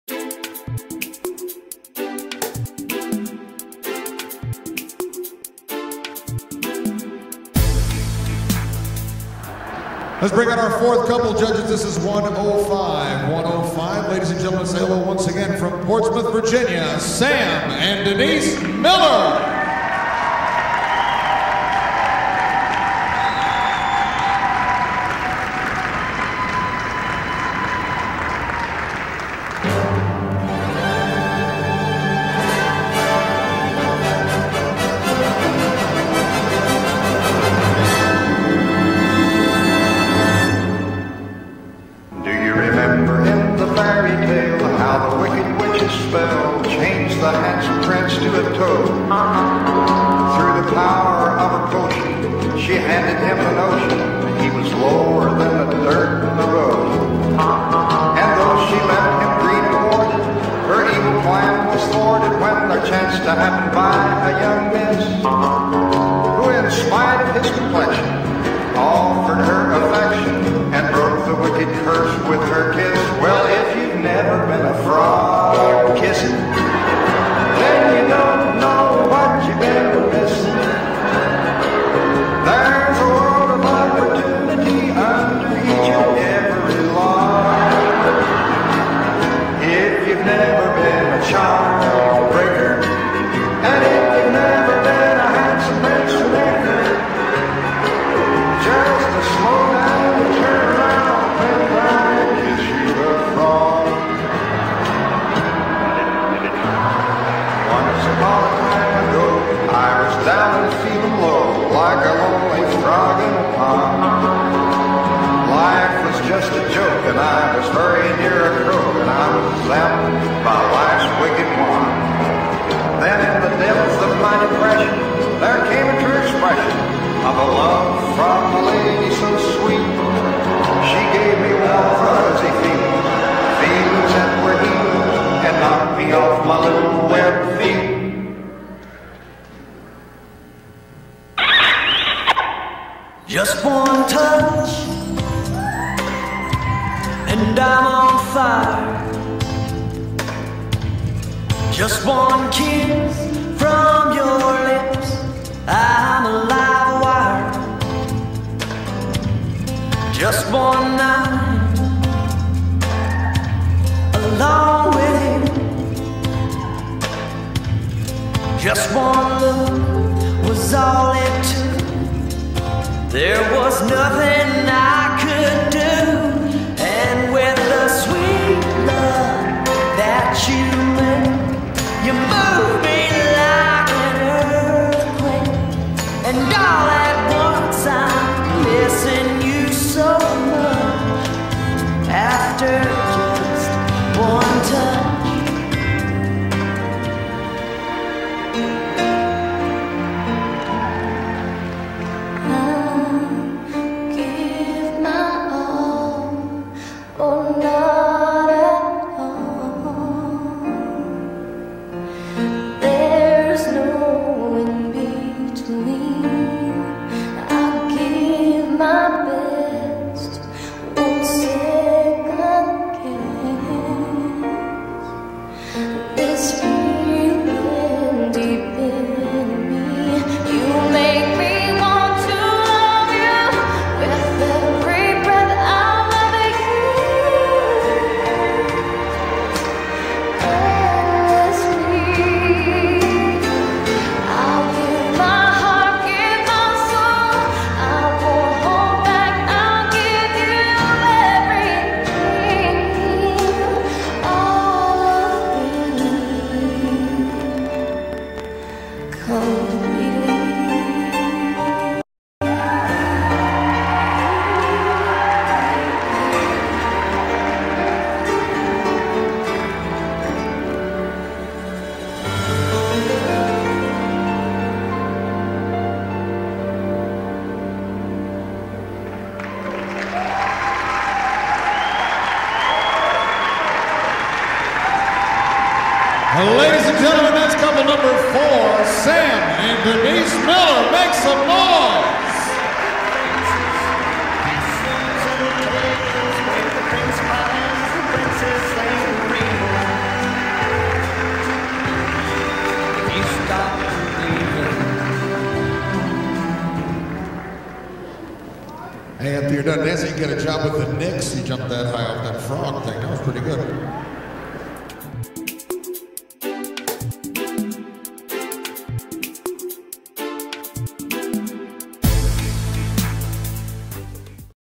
Sam Miller | Denise Miller | Solo Medley | Showcase Masters | 2003 UCWDC World Championships
Sam Miller | Denise Miller | Solo Medley | Showcase Masters | 3rd Place Overall | 2003 UCWDC World Championships | Nashville, Tennessee | Thank you, Kristin Geil for providing the video